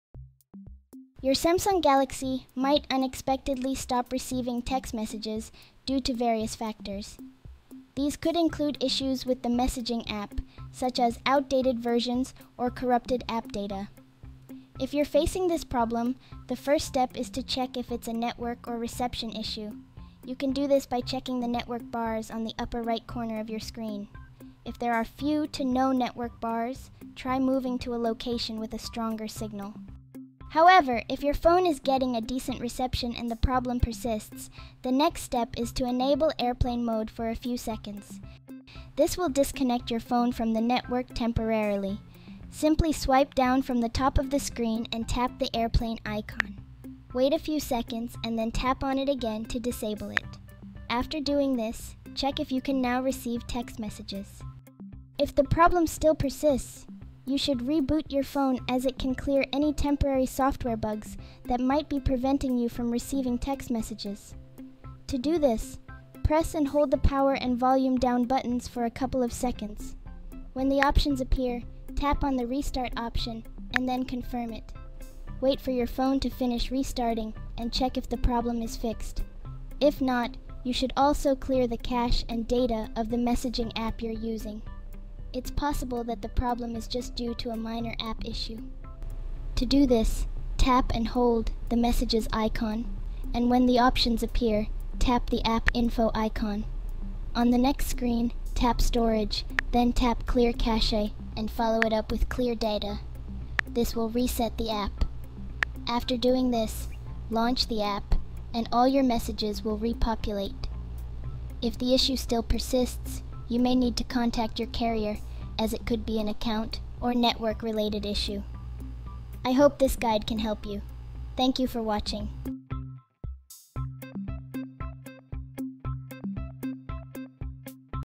How To Fix A Samsung Galaxy Phone That Can’t Receive Text Messages / SMS (2024)
In this video, you'll learn what to do if your Samsung Galaxy suddenly stops receiving text messages, which can be caused by various factors. If you're experiencing this issue, start by checking your network or reception status by looking at the bars on the upper right corner of your screen. If you still face problems, try enabling airplane mode momentarily, rebooting your phone, or clearing the cache and data of your messaging app to resolve the issue. If all else fails, reaching out to your carrier might be necessary to address potential account or network-related issues.